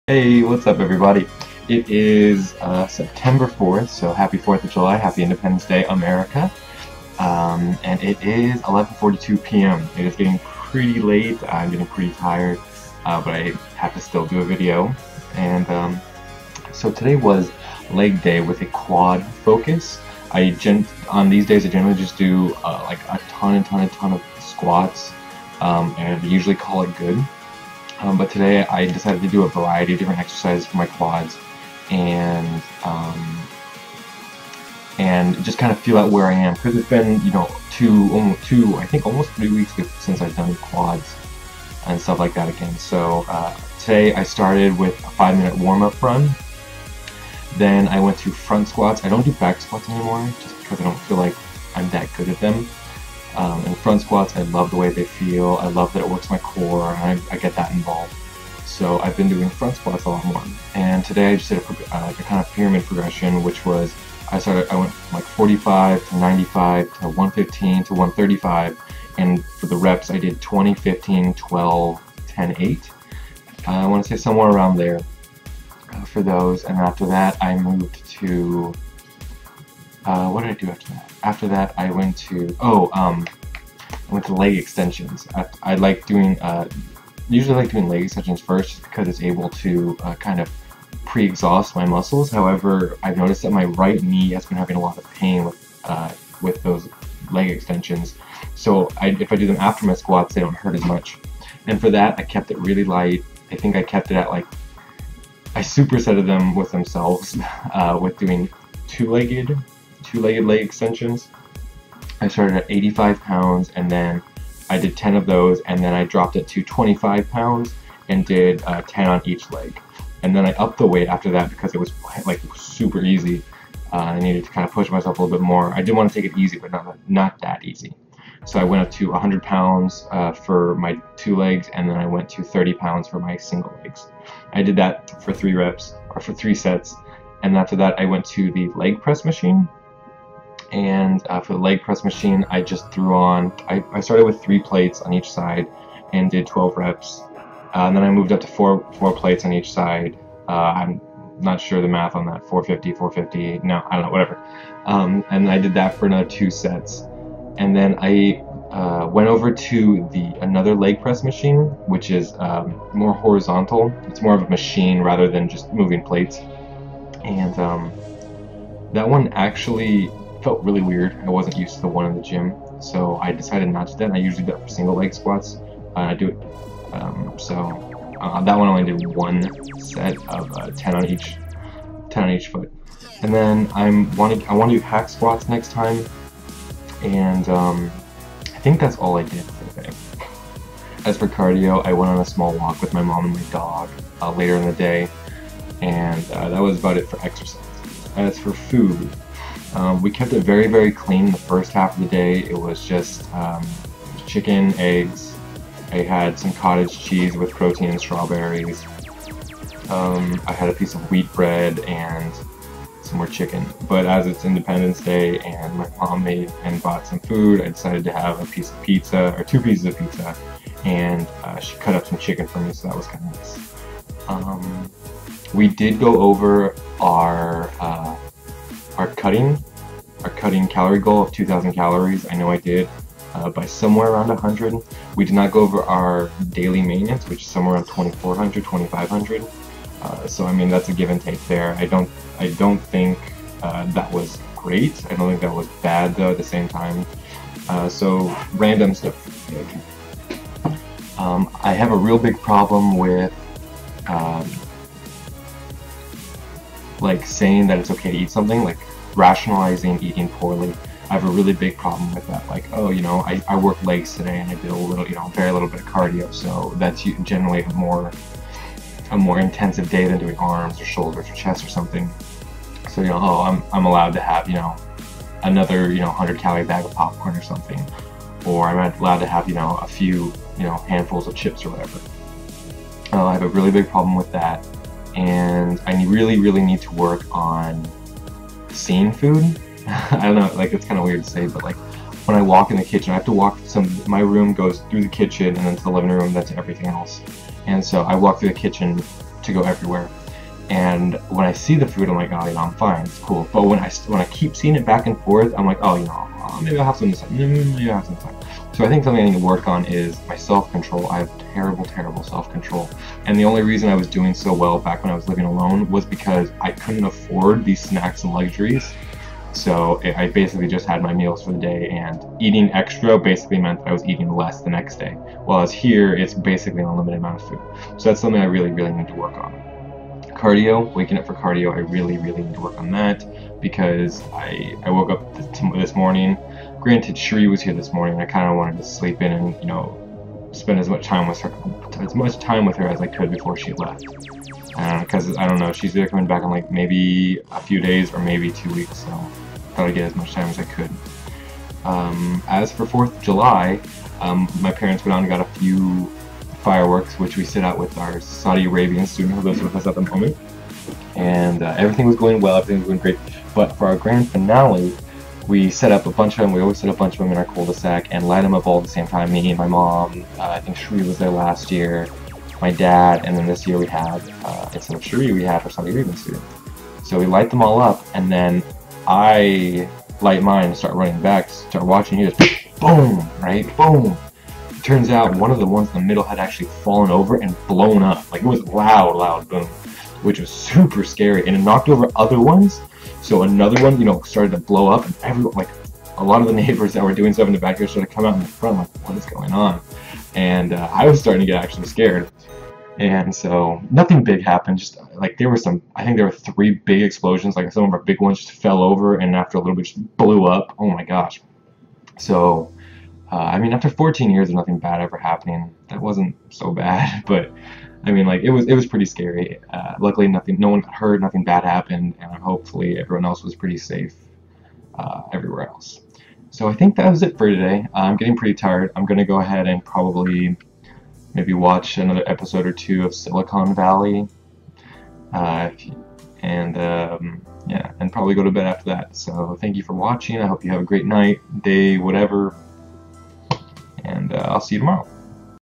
Fitness Vlog Day 6: Quads
From shit to fit. Independence Day EXPLOSIONS

Stats:
Weight: 202 lbs
Last BF%: 17